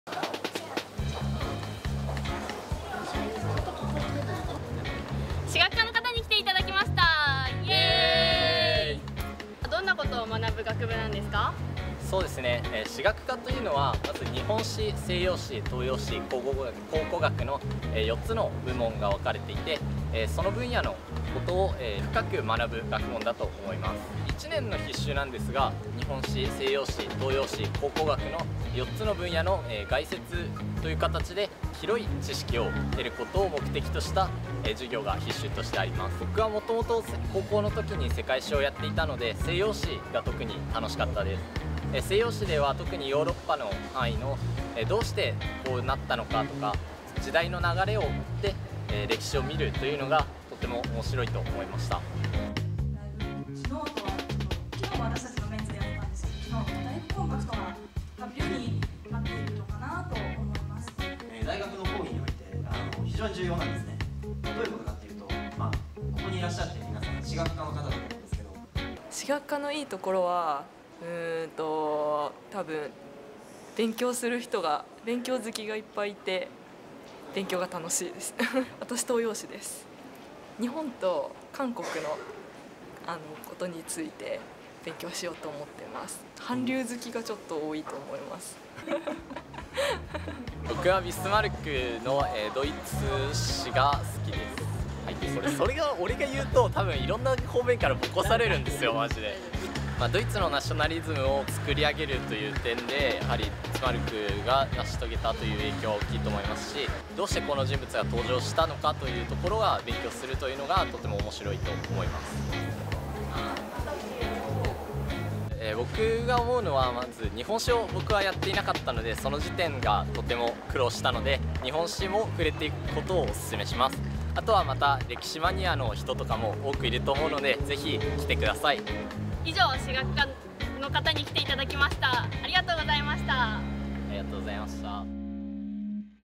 青山学院大学オープンキャンパス「文学部史学科」
オープンキャンパスで聞きました
文学部史学科ってどんな学部？